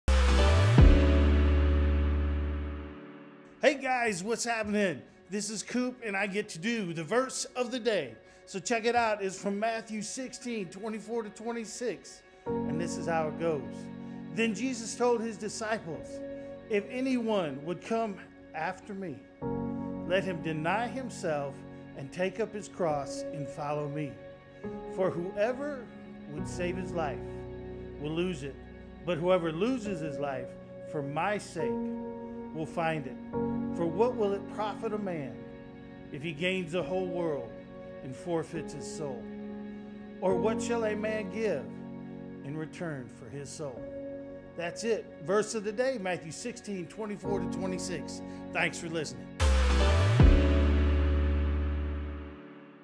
Verse of the Week - 8-23-20 - Matthew 16:24-26
Matthew 16:24-26

For whoever would save his life will lose it, but whoever loses his life for my sake will find it. For what will it profit a man if he gains the whole world and forfeits his soul? Or what shall a man give in return for his soul?